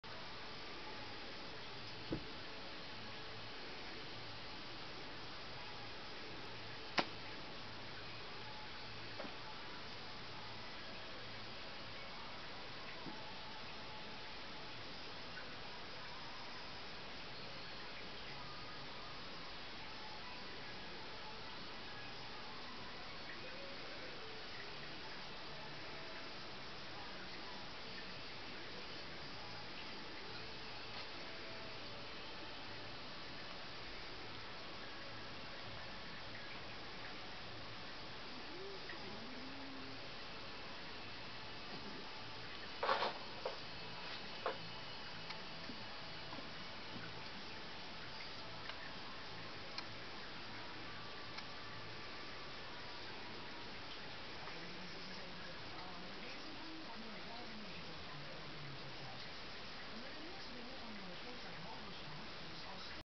DSCF2619 red bandita whit hear grand father bagera
cfa ch/int ch/ont pr Bagera Sabya of Shanna playing whit his grand daughter Shanna's Red Bandita